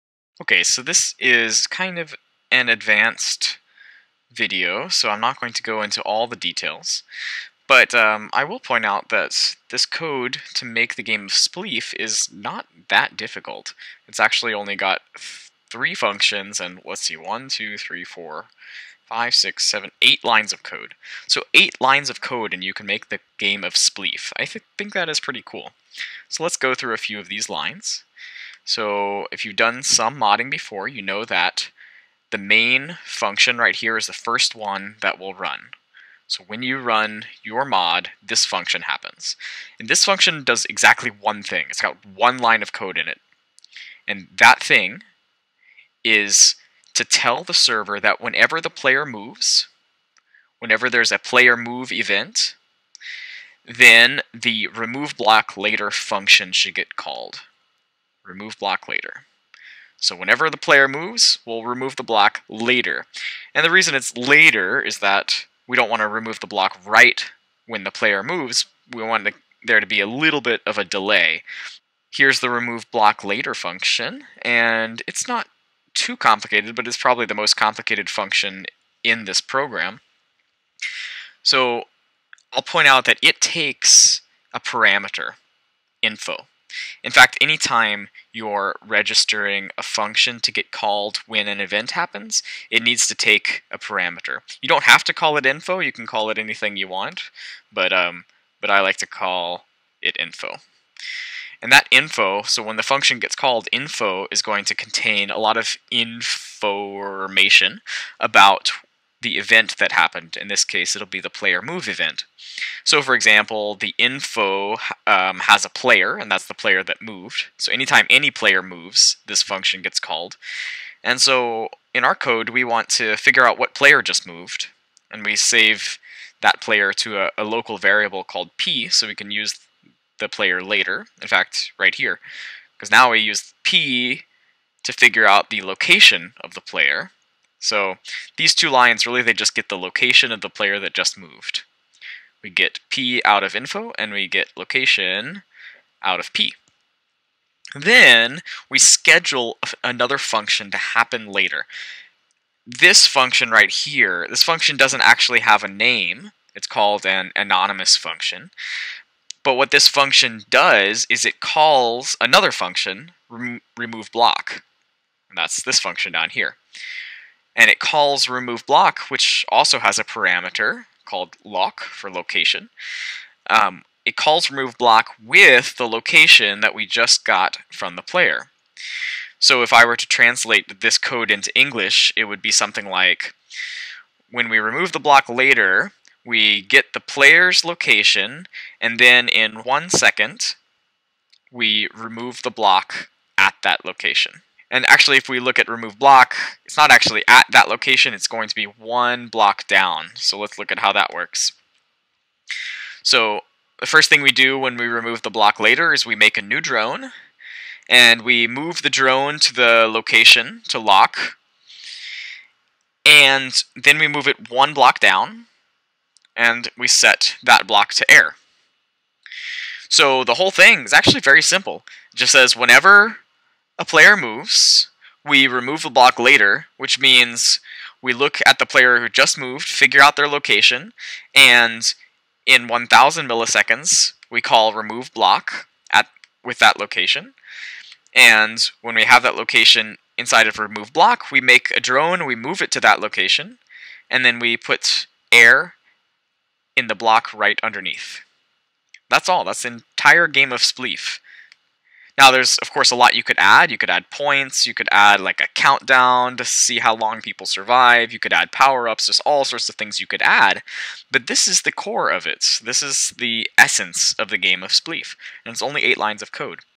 Spleef Modding Tutorial (Part 2)
In this series, we show how you can code your own game of Spleef.  Coding your own versions of existing games is a great way to learn how to code.

In this video, we look at the code for Spleef (which is only 8 lines long!).  In the previous video, we saw how to create the arena.